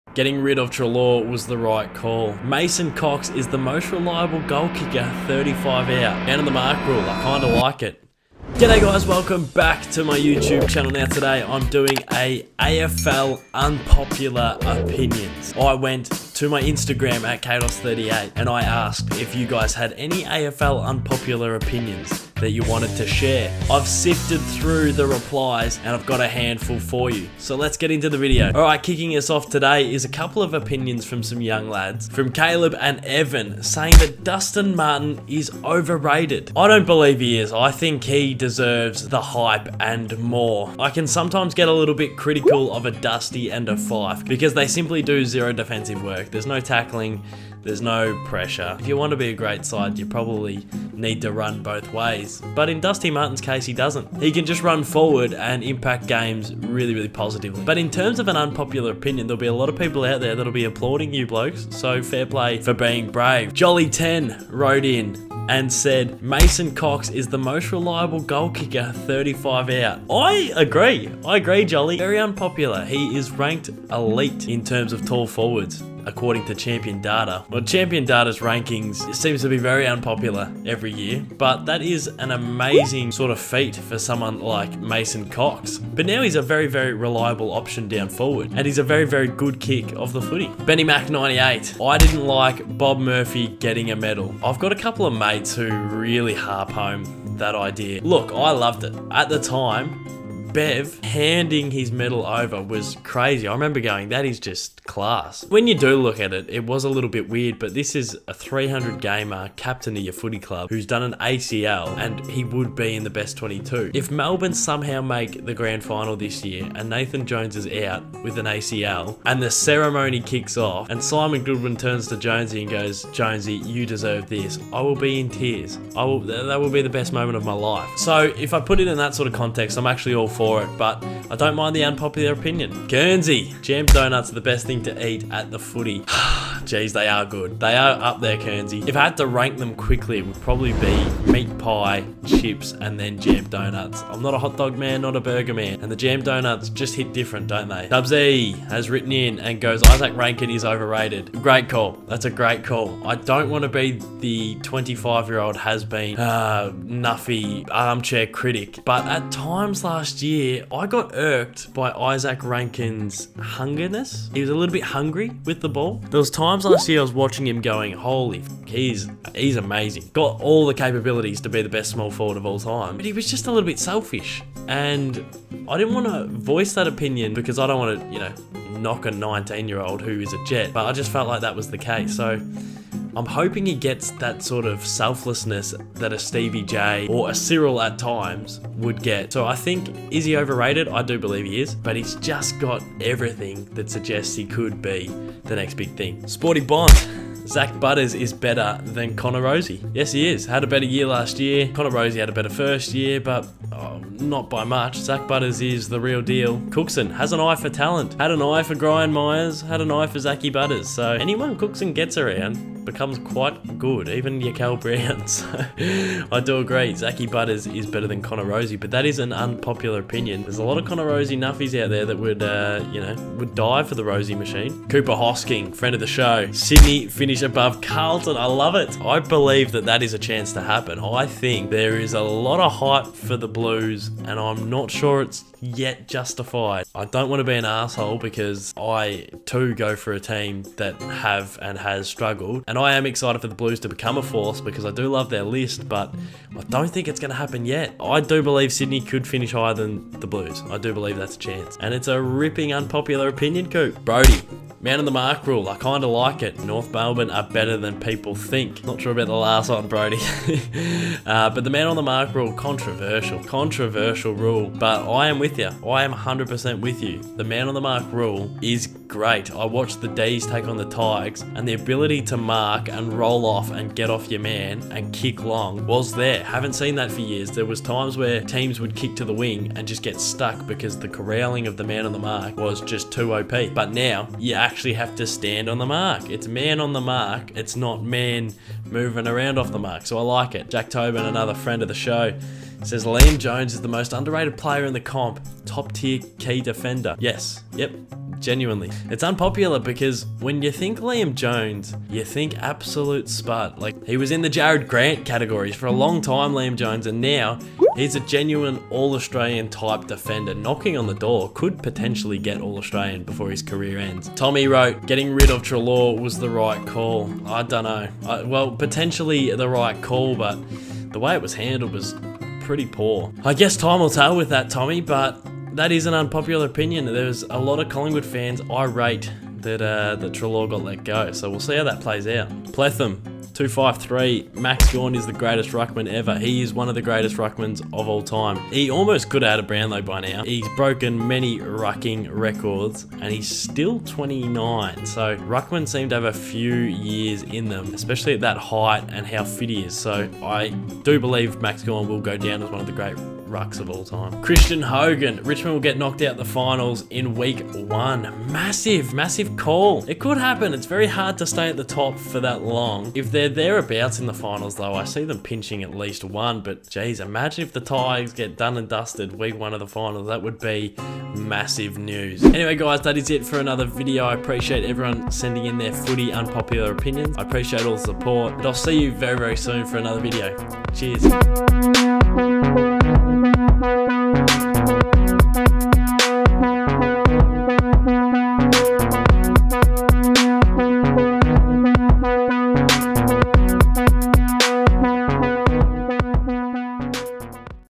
Unpopular Opinions (AFL)
Tackling AFL's biggest Unpopular Opinions, sent in by you.